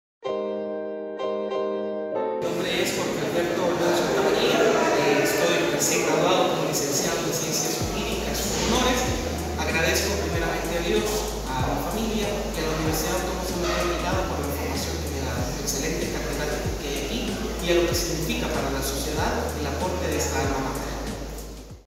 Jorge Alberto Olmedo
Entrevista a Jorge Olmedo
El viernes 3 de mayo se llevó a cabo la graduación de cuatro estudiantes graduados con Cum Laude de la carrera de Licenciatura en Ciencias Jurídicas de la Facultad de Jurisprudencia y Ciencias Sociales en el Aula Magna del edificio 3 campus II, de la Universidad Dr. José Matías Delgado.